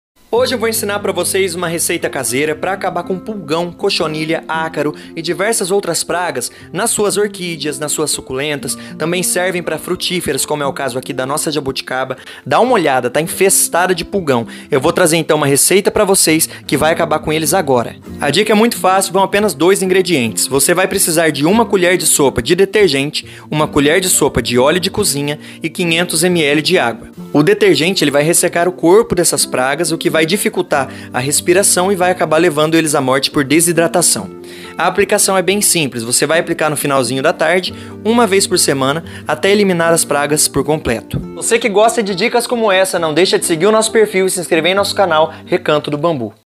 O melhor inseticida caseiro para pulgão cochonilha lagarta e outras pragas em todas as suas plantas
nesse vídeo ensino o melhor inseticida caseiro natural para controle de pragas como pulgões cochonilhas ácaros lagartas mosca branca percevejos e muitos outros, serve também para controle da larva minadora que causa a folha enrugada nos Limoeiros e laranjeiras.